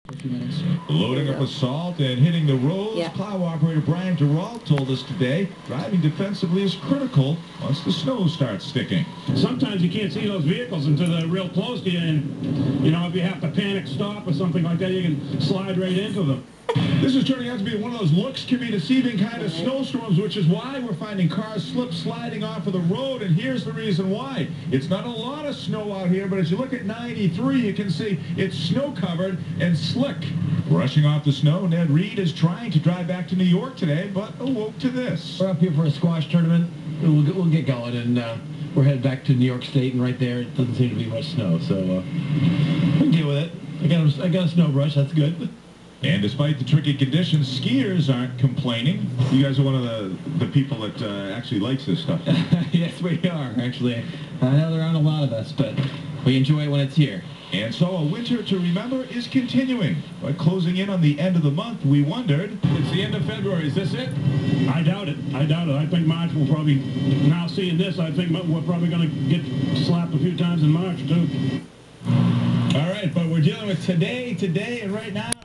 Uncle Brian 2.27.11.AVI
Brian Dureault being interviewed by Channel 7 News WHDH - February 27, 2011 while out plowing in Andover on I93 during one of the many snow storms to hit New England this winter.